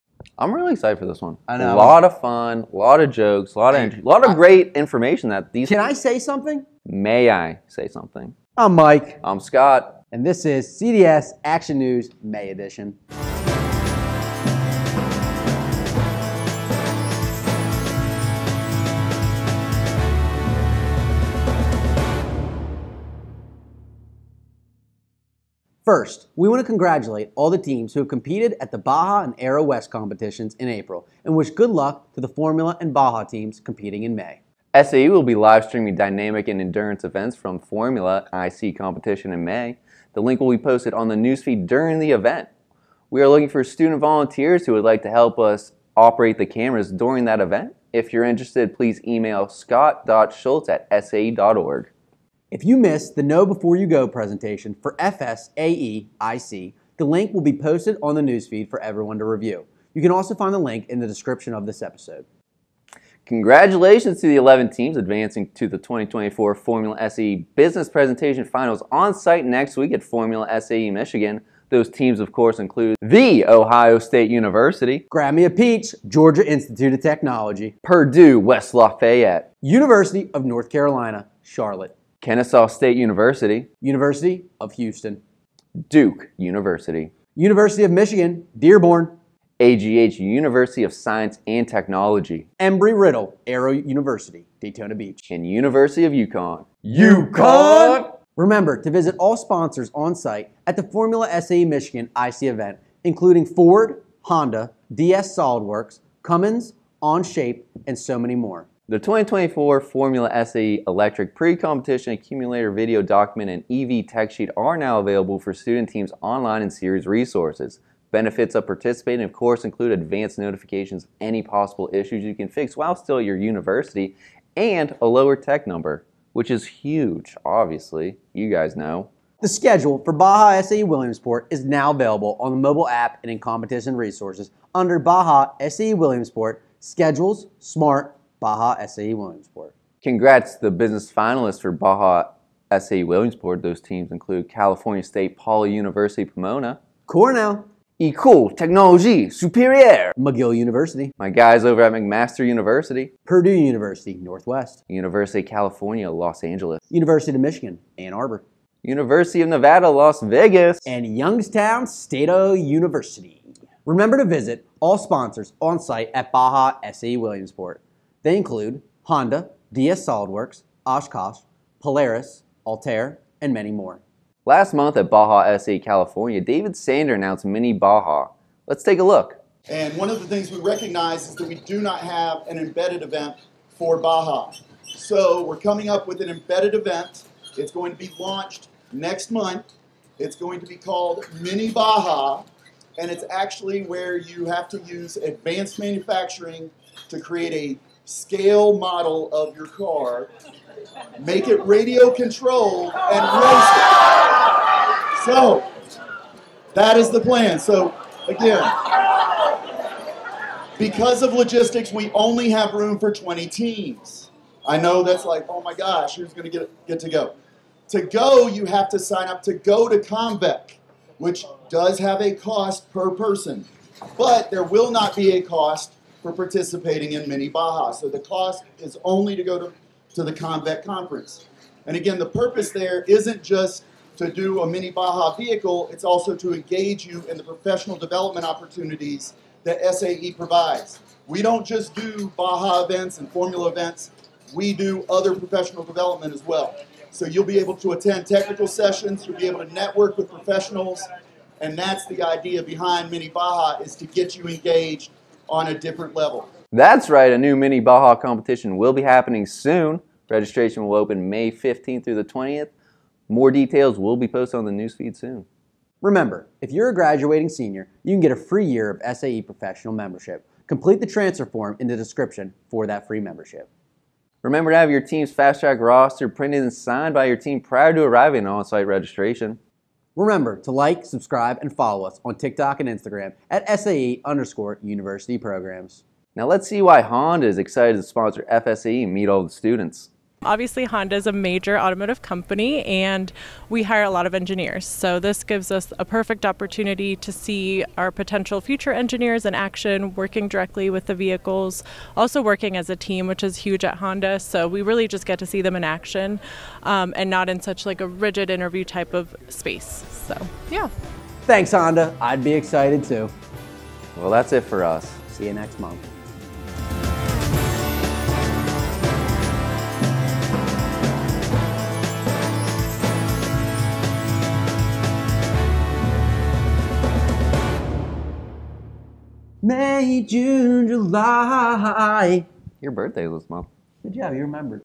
CDS Action News May 2024
Intro 0:00
FSAE Livestream 0:34
FSAE IC Know before you go 0:52
FSAE Business Finalists 1:02
Sponsors 1:38
Formula EV Accumulator 1:48
Baja SAE Williamsport schedule 2:09
Baja SAE Williamsport Business Finalists 2:19
Sponsors 2:43
Mini Baja Competition 2:54
Free SAE Membership 4:25
Fast track roster 4:34
Social media 4:39
Honda at Formula SAE 4:46